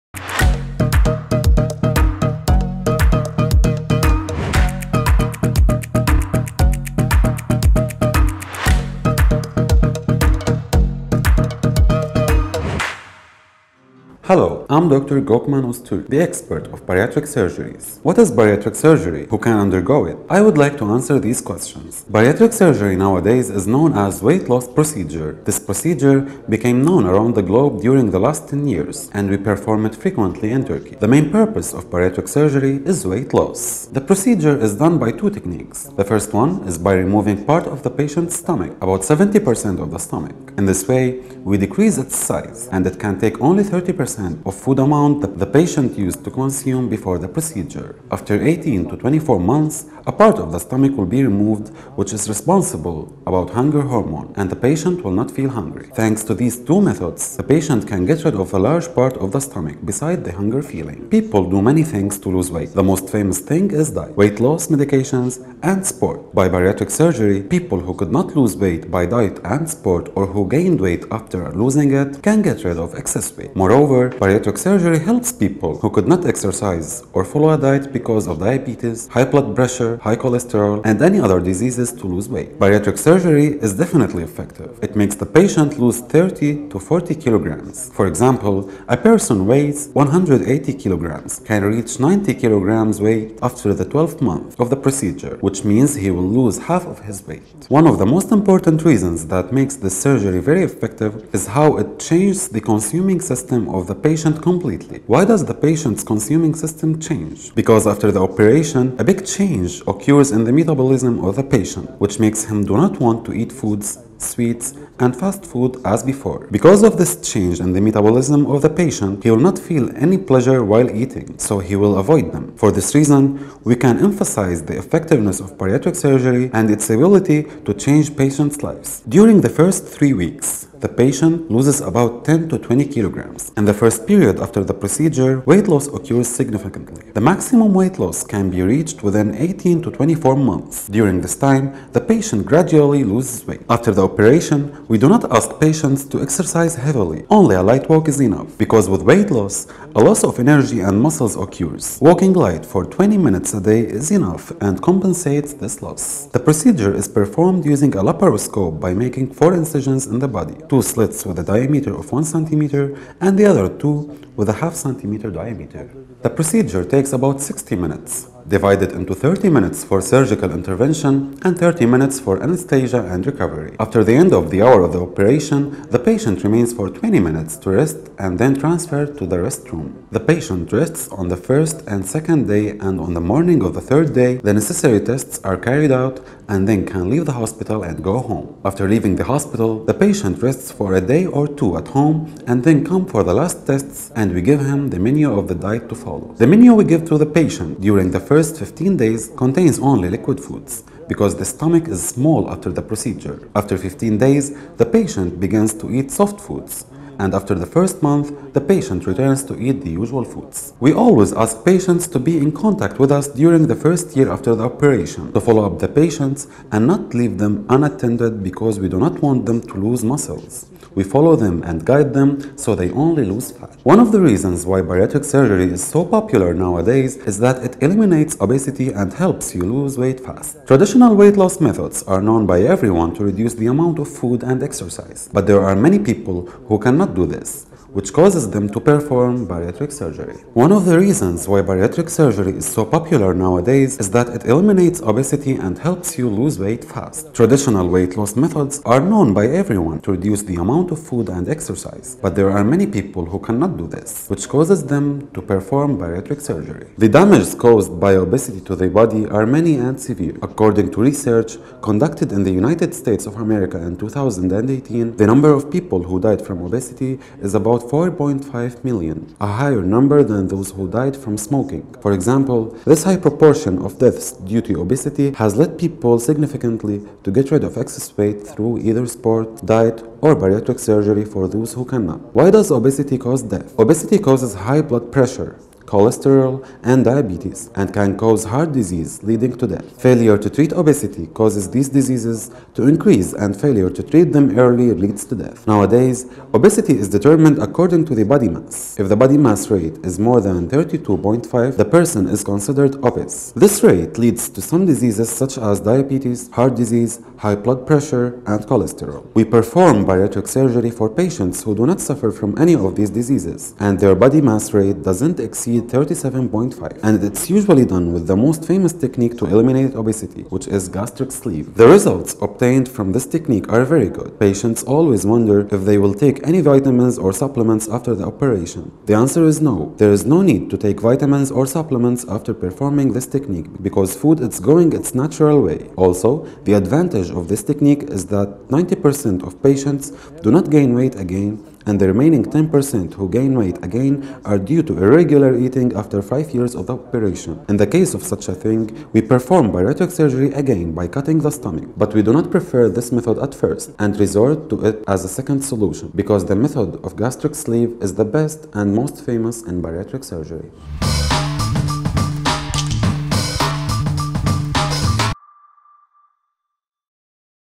Lose 80 kg in 12 Months with Bariatric Surgery
People suffering from morbid obesity are usually advised to lose weight as soon as possible by experts. Fast and healthy weight loss can be achieved by bariatric surgery accompanied by a specified healthy diet.

Since obesity poses a serious threat to the health and well being of individuals losing weight as fast and healthy as possible is crucial in averting the health risks of obesity. Especially morbid obese people may need bariatric surgery since they are under constant risk.

In today’s video, our doctor Gökmen Öztürk will talk about this issue and he will talk about the benefits and specifics about bariatric surgery.

Bariatric surgery is considered to be one of the fastest, most accurate and safest solutions for overweight and obese people.

In a nutshell, this informative video is a must-watch for overweight people and people who are suffering from obesity.